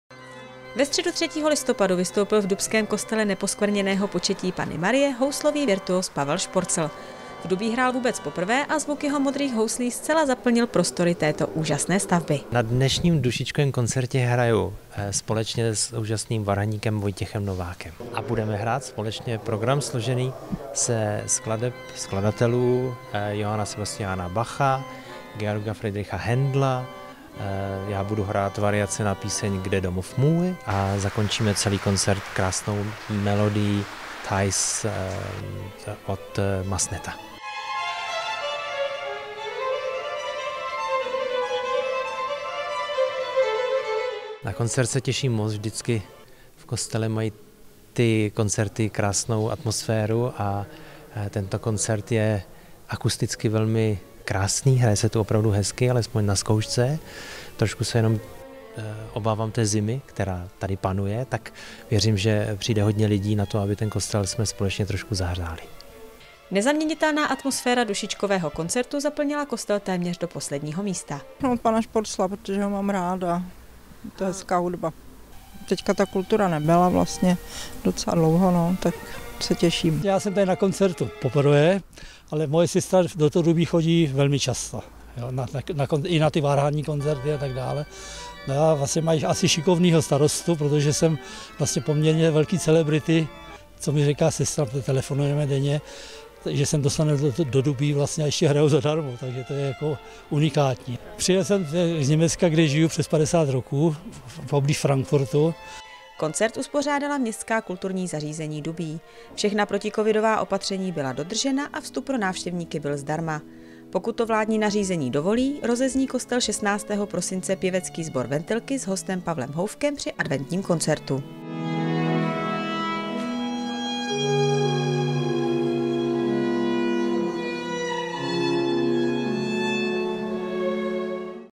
DUBÍ: Houslový virtuos Pavel Šporcl vystoupil na koncertě v Dubí
Ve středu 3. listopadu vystoupil v dubském kostele Neposkrveněného početí Panny Marie houslový virtuos Pavel Šporcl. V Dubí hrál vůbec poprvé a zvuk jeho modrých houslí zcela zaplnil prostory této úžasné stavby.
Nezaměnitelná atmosféra dušičkového koncertu zaplnila kostel téměř do posledního místa.
Koncert uspořádala Městská kulturní zařízení Dubí. Všechna protikovidová opatření byla dodržena a vstup pro návštěvníky byl zdarma. Pokud to vládní nařízení dovolí, rozezní kostel 16. prosince pěvecký sbor Ventilky s hostem Pavlem Houfkem při Adventním koncertu.